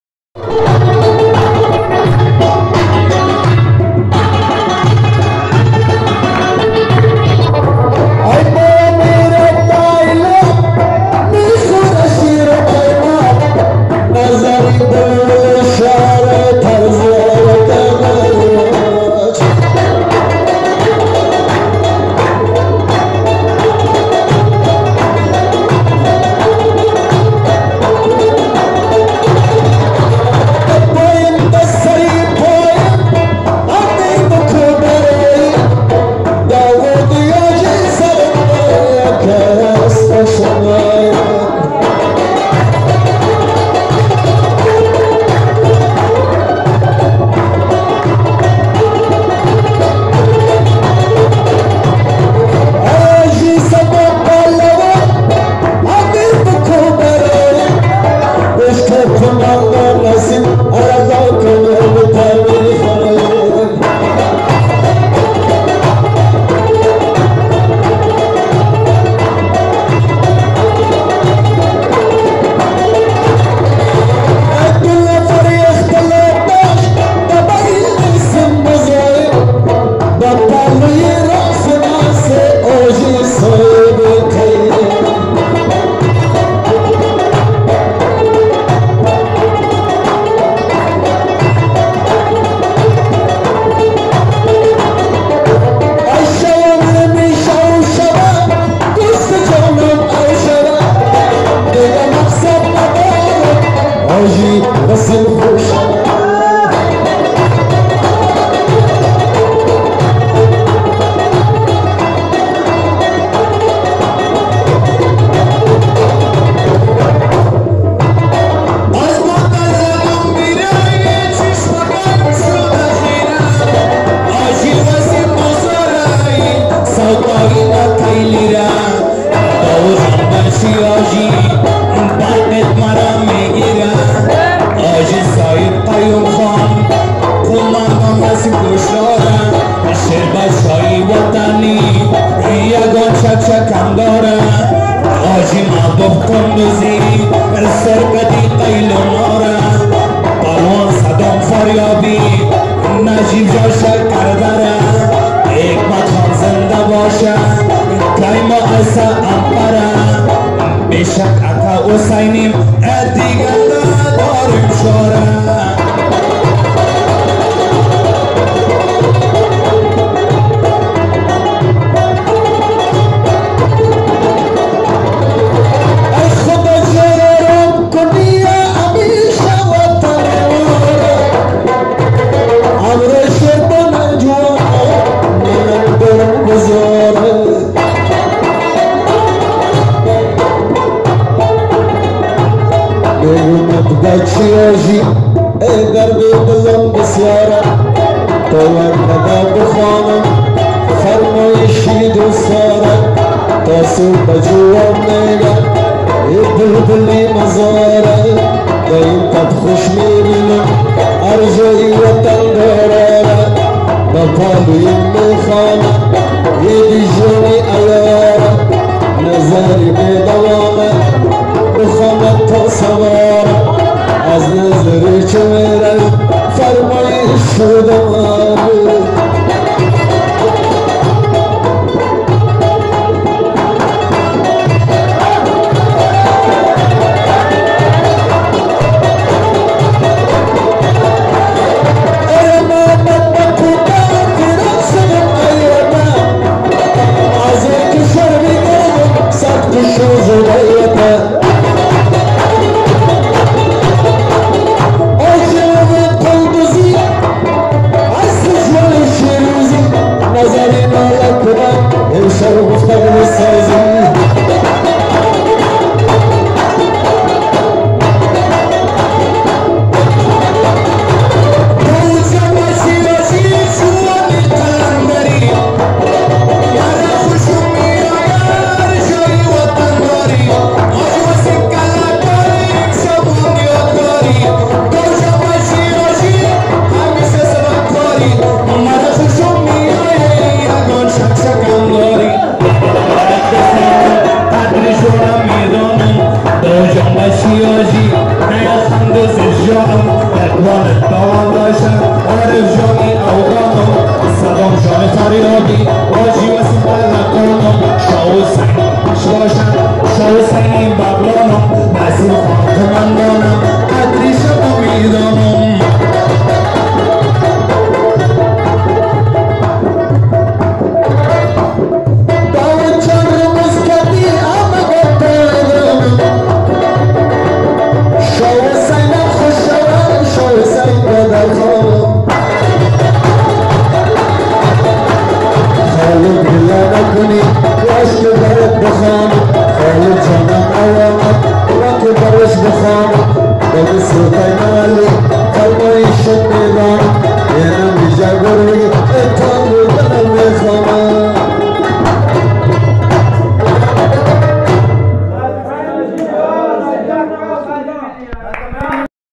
حاجی گل محمد نظری ، فیروز کندوزی، نعیم شیخعلی و عید محمد عیار (مجلس تهران)
حاجی گل محمد نظری
 نعیم شیخعلی
 عید محمد عیار
 دمبوره
دمبوره جدید